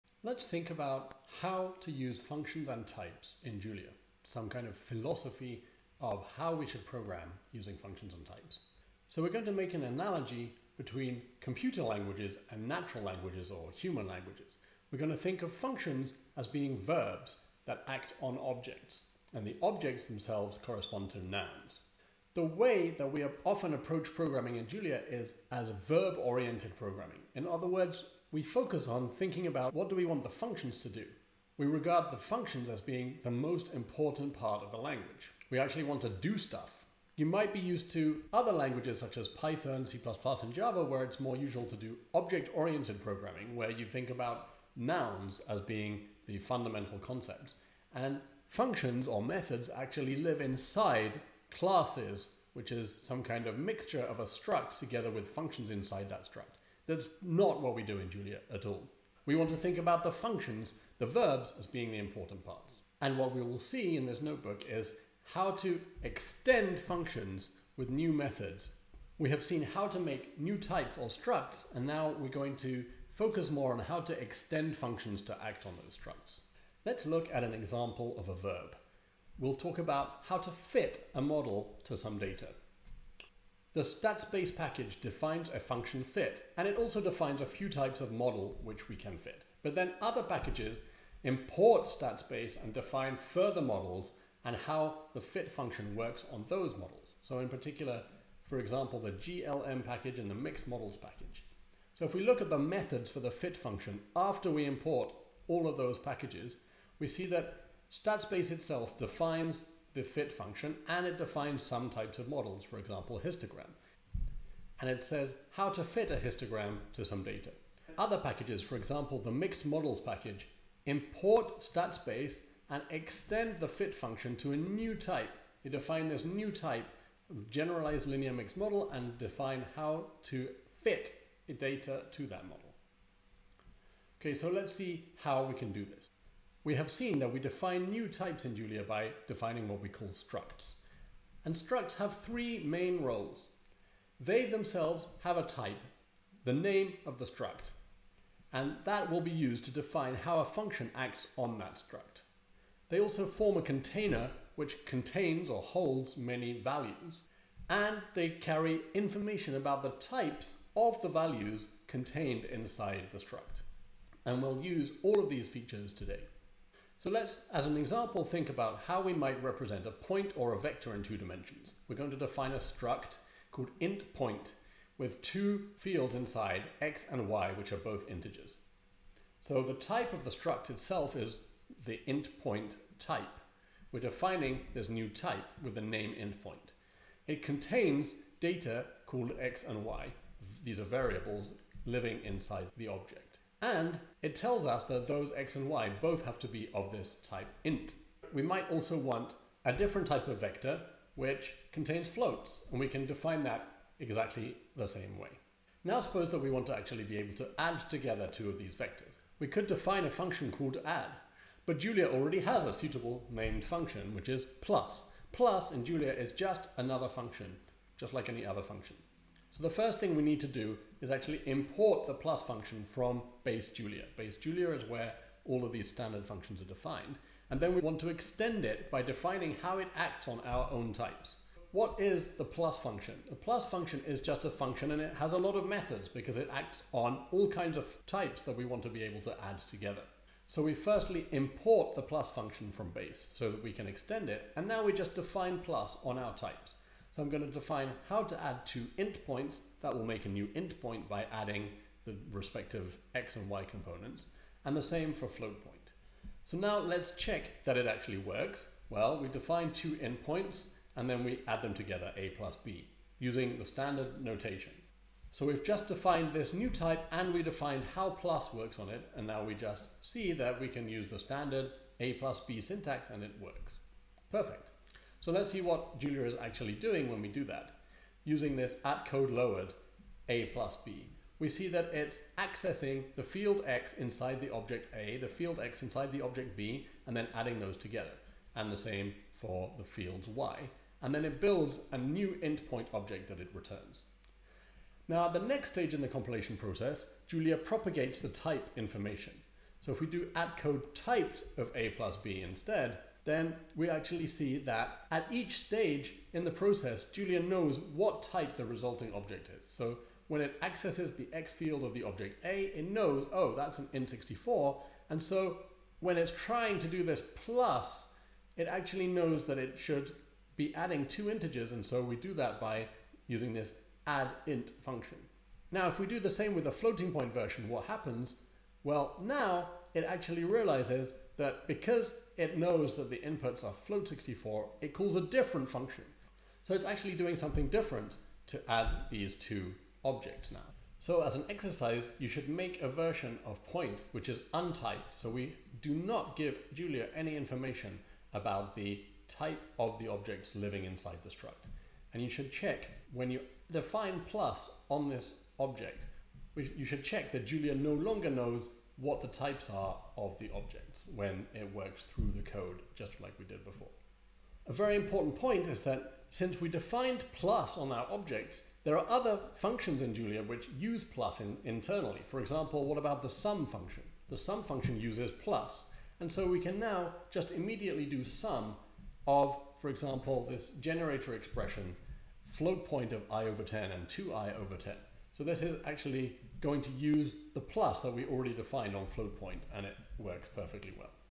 Verbs and nouns in Julia | Week 5 | 18.S191 MIT Fall 2020
In Julia we focus on the actions of verbs, i.e. functions.
One of the purposes of objects (nouns) is to define different ways that verbs can work.

We'll continue this story next week!

00:00 Welcome!
00:10 Help us add time stamps or captions to this video! See the description for details.